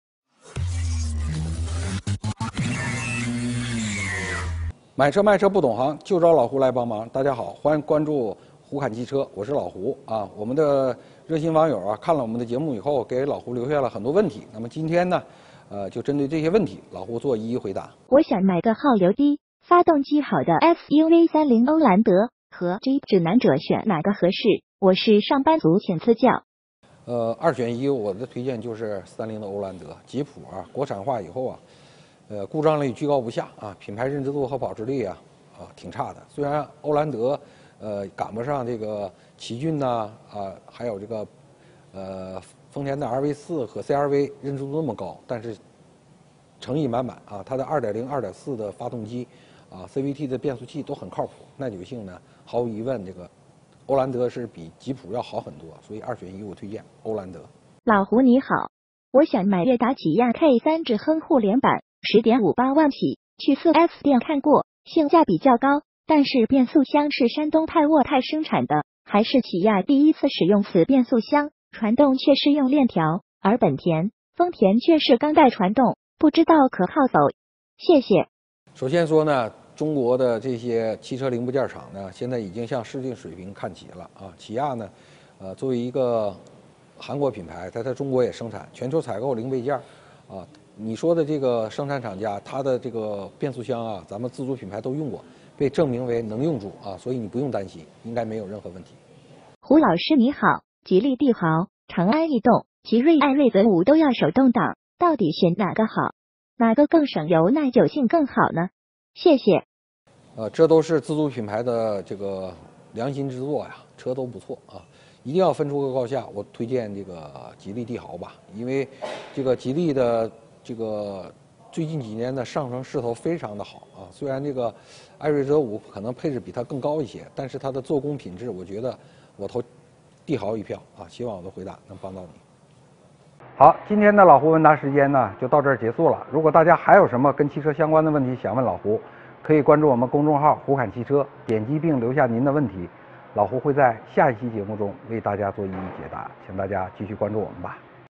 上班族买车，欧蓝德和指南者选哪个？首选它质量更稳定
起亚K3质量稳定吗？和日系比怎么样？老胡为你分析答案
车系： 帝豪 欧蓝德 指南者 起亚K3